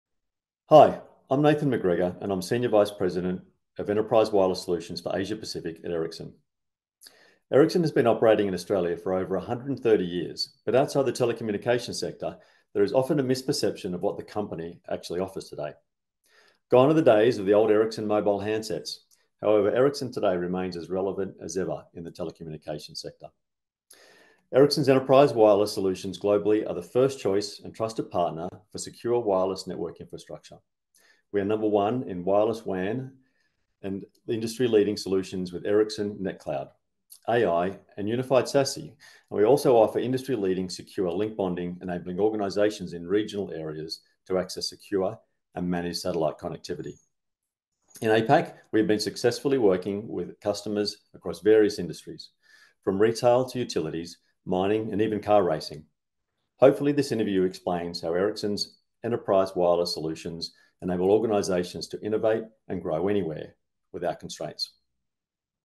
Ericsson's Nathan McGregor
Nathan McGregor, Senior Vice President of Enterprise Wireless Solutions for Asia Pacific at Ericsson, introduces the company's longstanding presence in Australia and addresses common misconceptions about Ericsson's modern capabilities beyond its historic mobile handsets. He highlights how Ericsson has evolved to become a leading force in secure wireless networking infrastructure, recognized globally for its Wireless WAN and industry-leading solutions such as Ericsson nCloud AI and unified SASE, as well as secure link bonding that enables organizations, even in regional areas, to access and manage satellite connectivity. Nathan explains how Ericsson continues to power innovation across diverse sectors including retail, utilities, mining, and even car racing, demonstrating how their enterprise wireless solutions support organizational growth and innovation without boundaries.